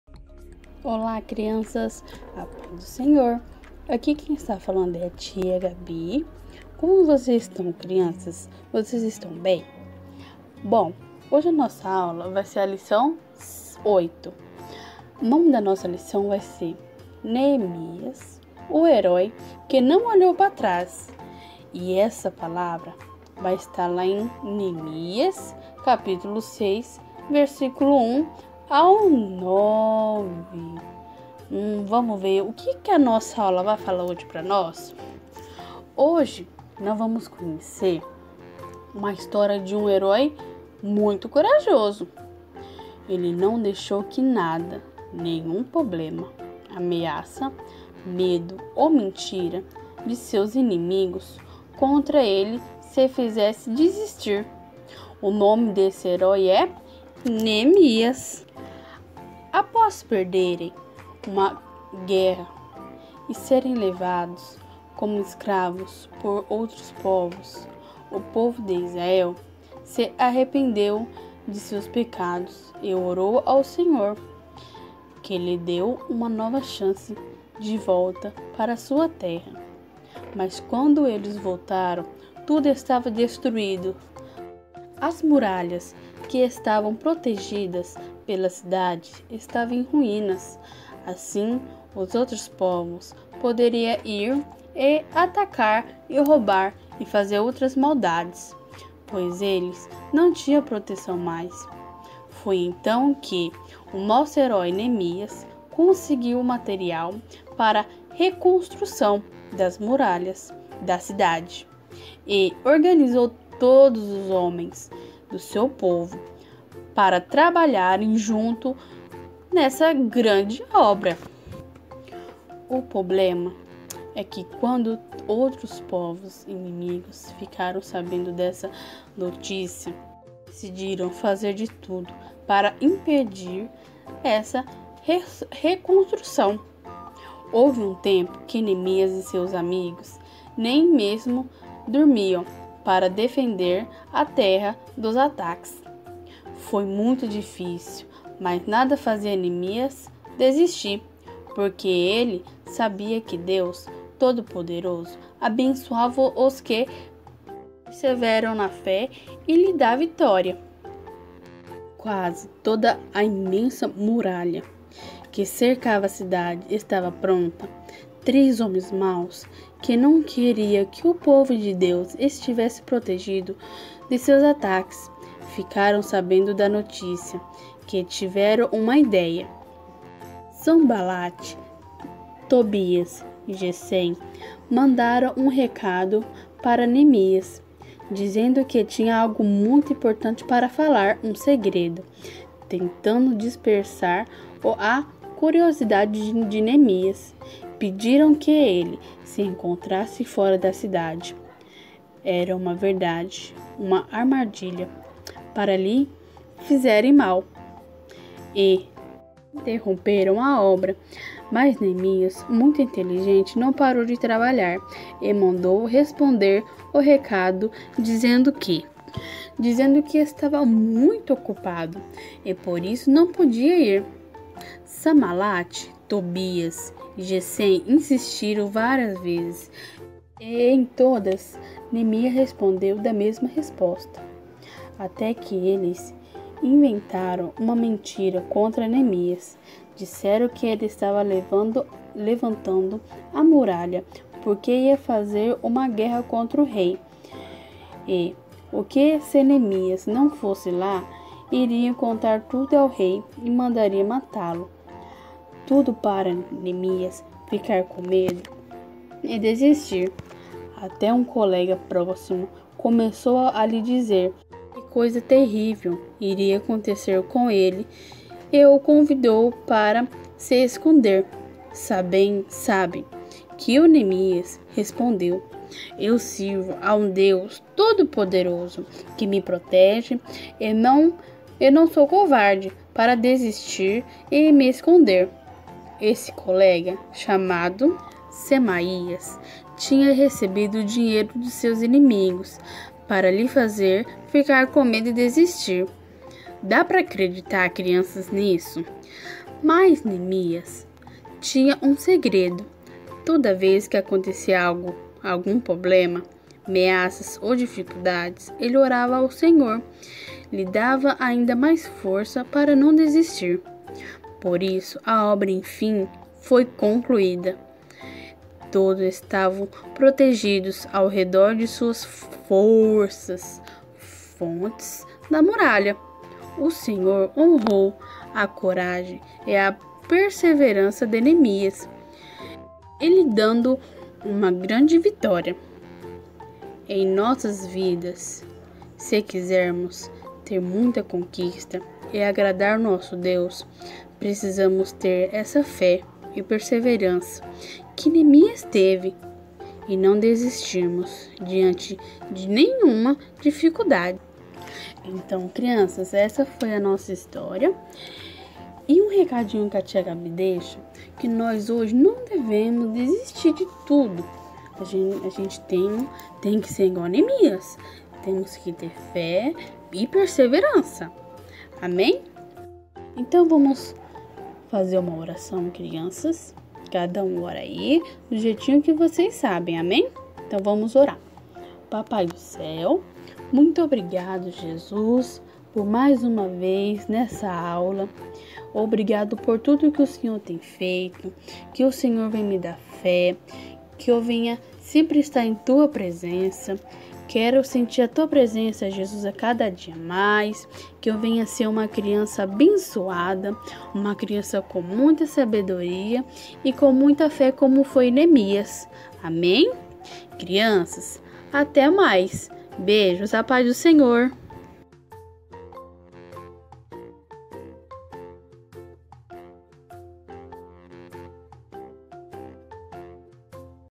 Neemias - Tia Gabi
CPAD - Lição 8 Juniores
Neemias, o Herói que não olhou para trás.
PROJETO EBD ONLINE IEAD Paraíso do Norte, PR.